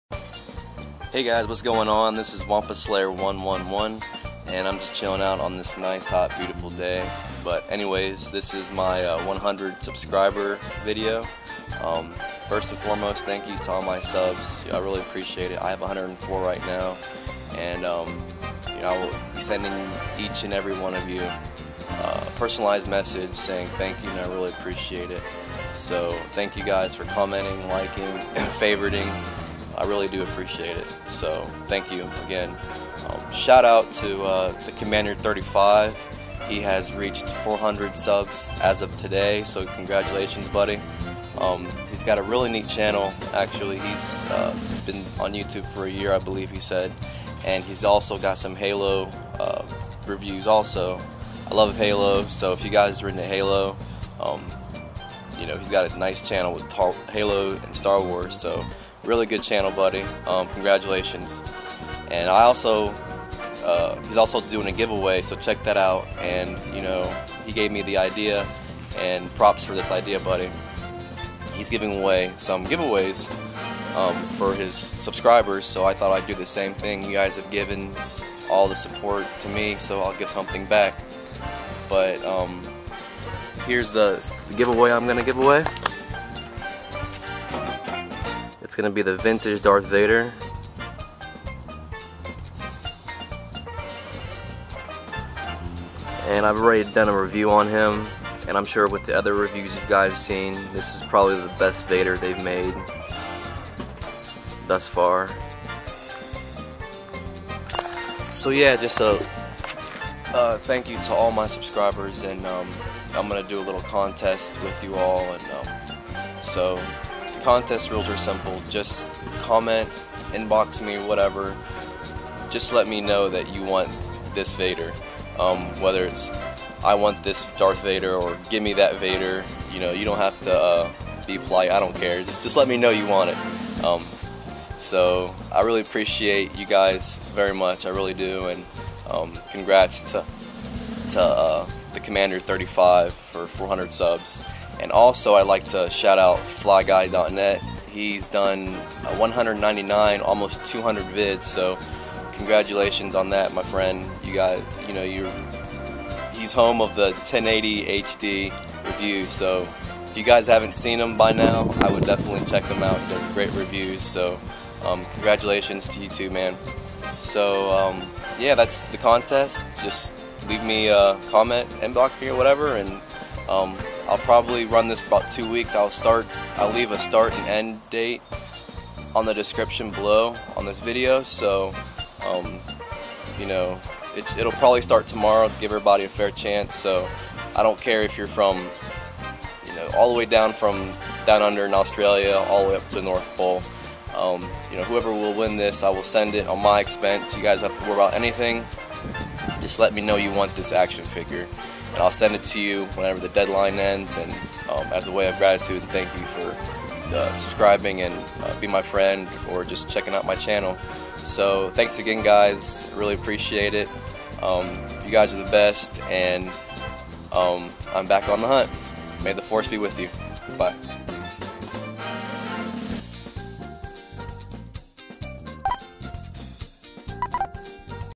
100+ subs video and contest giveaway!!!!
the contest will start  Thursday, june 2nd and end Friday, june 17th!!! I Will post a video on the 17th to draw names out of a hat.